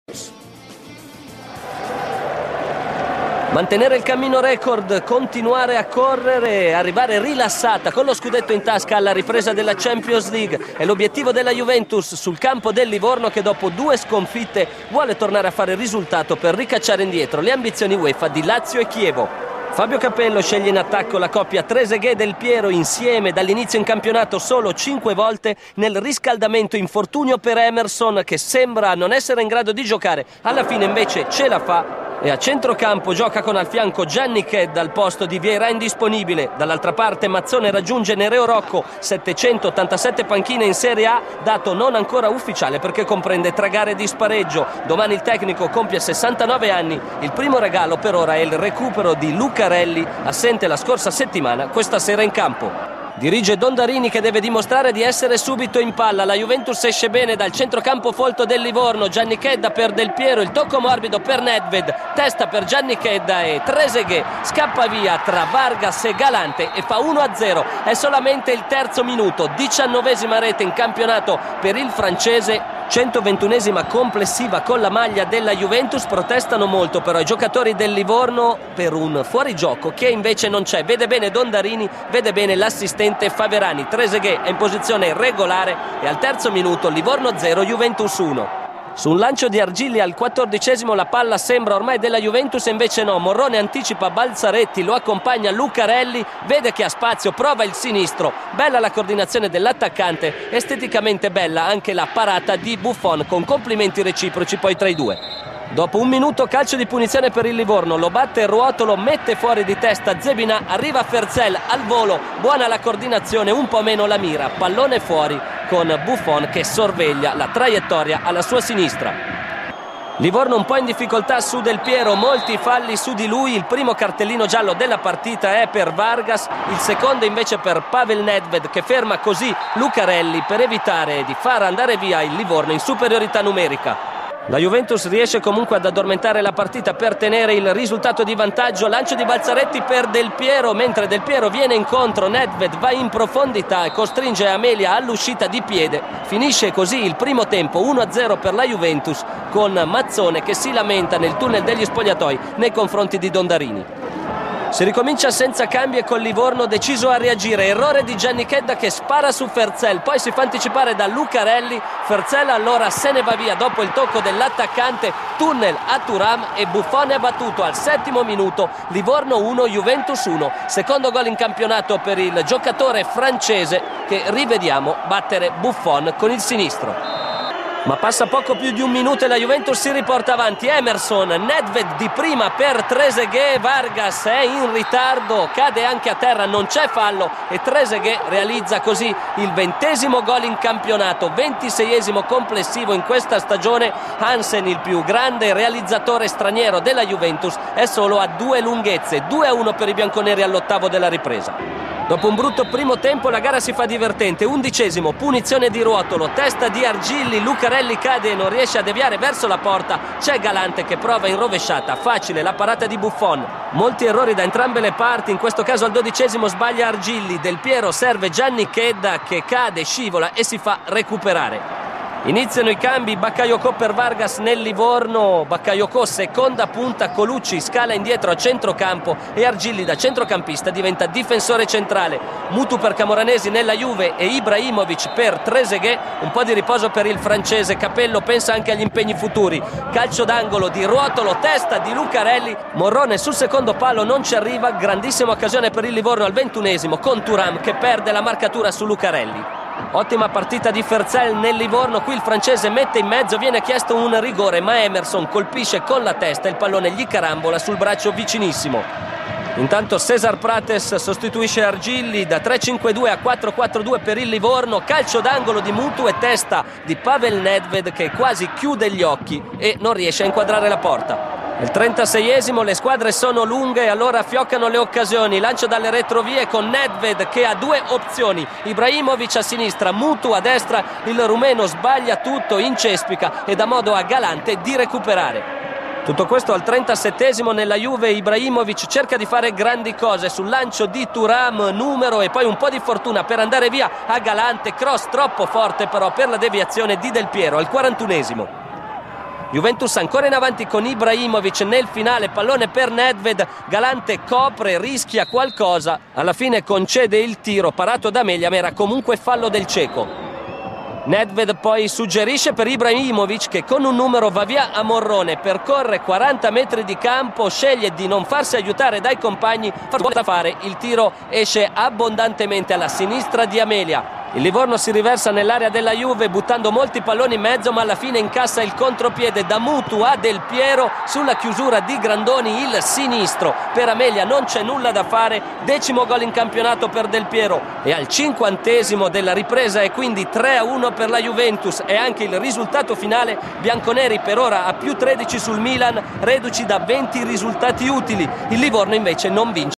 Livorno 1-3 Juventus - Campionato 2005/06
30° Giornata del Campionato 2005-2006

Goals :
3' D.Trezeguet (JUVENTUS)
52' M.Pfertzel (LIVORNO)
53' D.Trezeguet (JUVENTUS)
90' A.Del Piero (JUVENTUS)